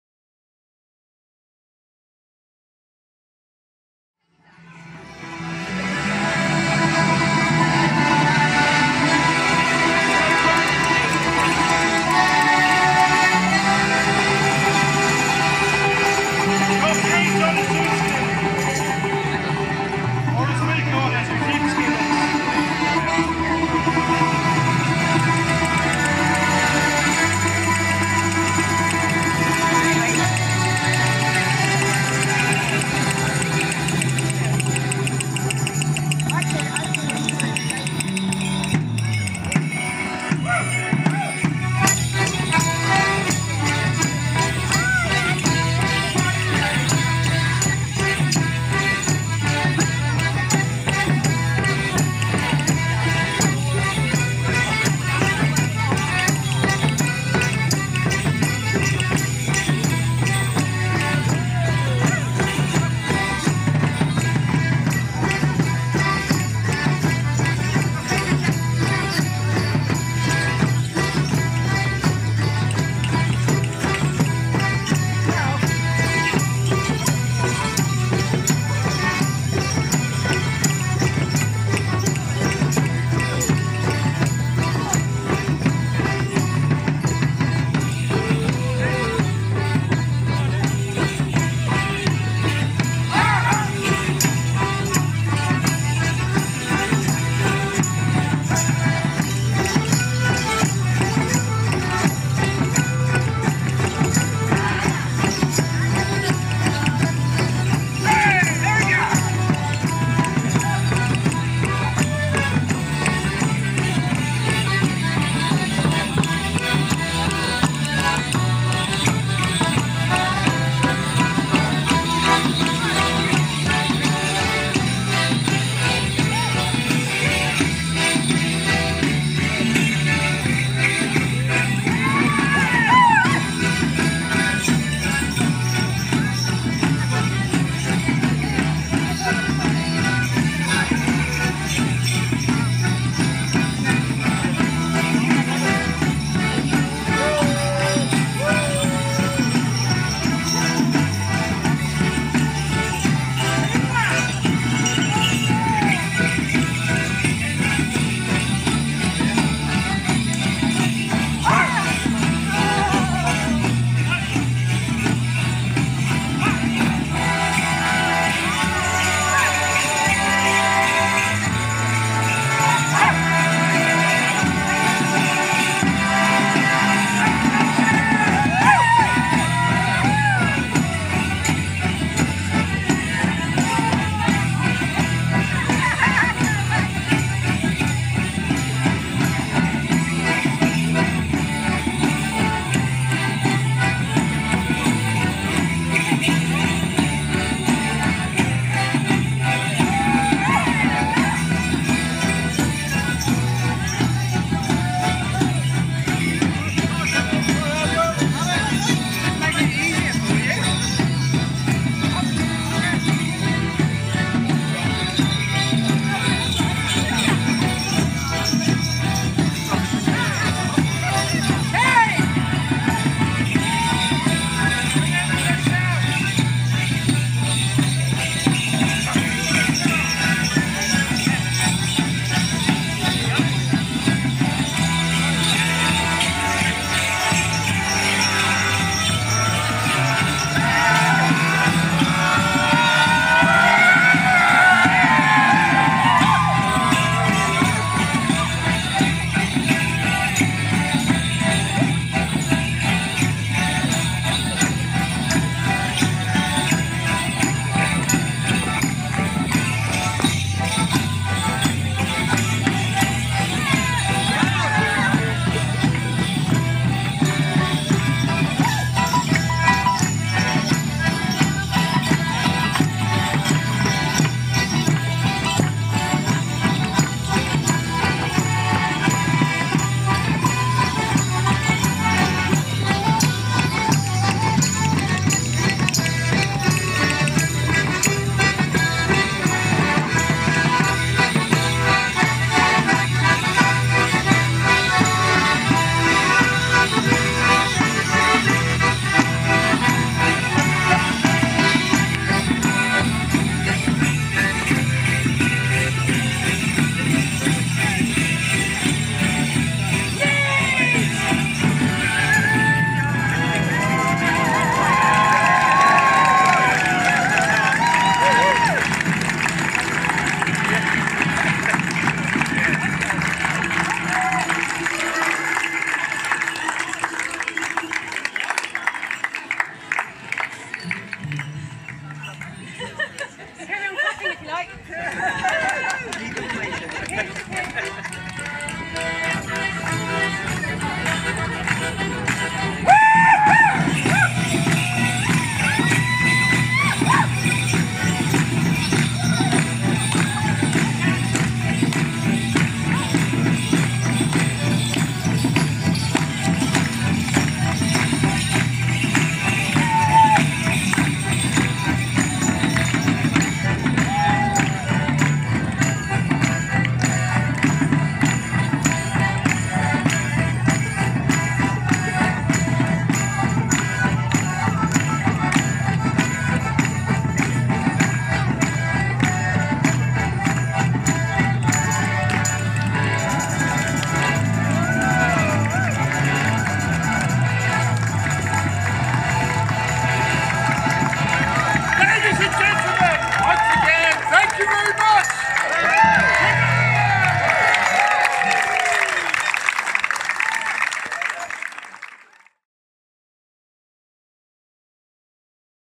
Sheep Skinners 2021
Sheep Skinners performed at Wreckers Day of Dance 2021, Cawsand, Cornwall.